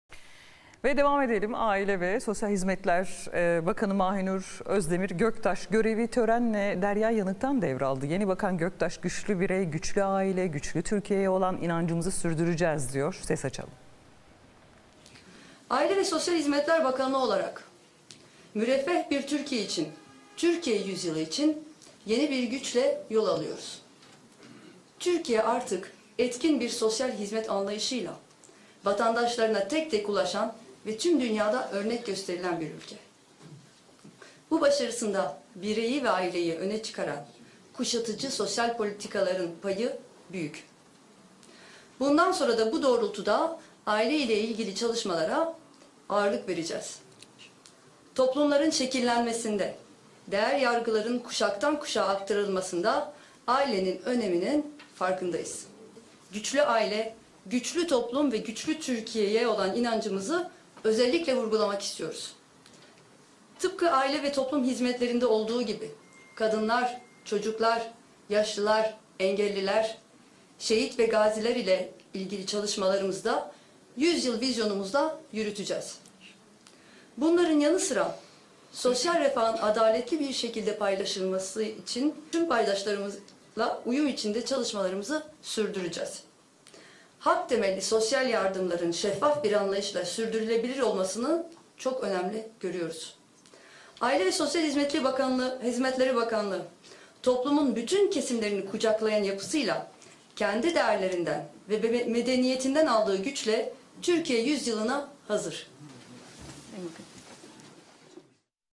Aile Bakanı Göktaş'tan ilk mesaj
Aile ve Sosyal Hizmetler Bakanı Mahinur Özdemir Göktaş, görevi törenle Derya Yanık'tan devraldı. Törende konuşan Göktaş, "Öncelikle depremin açtığı yaraları sarmak için bakanlığımızın büyük bir özveriyle çalıştığına şahit olduk. Bundan sonra da aynı şekilde çalışmaya devam edeceğiz" ifadelerini kullandı.